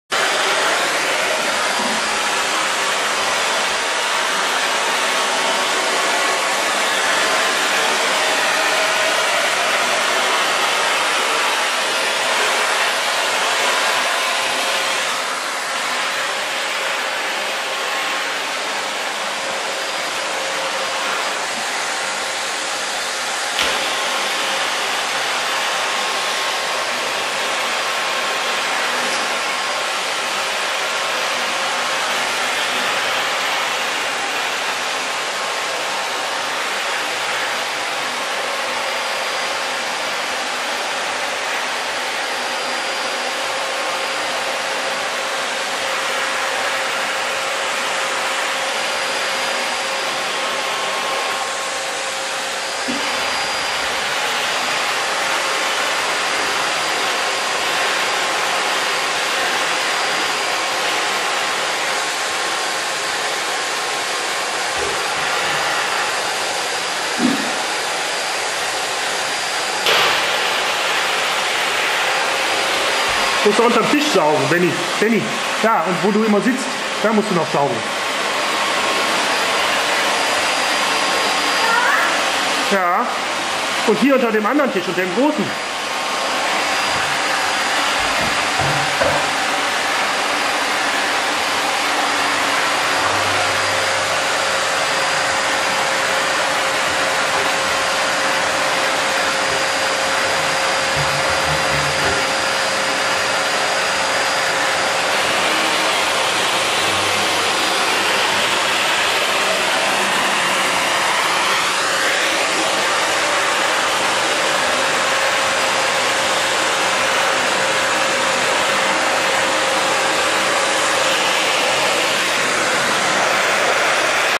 Ben der Putzmann
Ben saugt das Wohnzimmer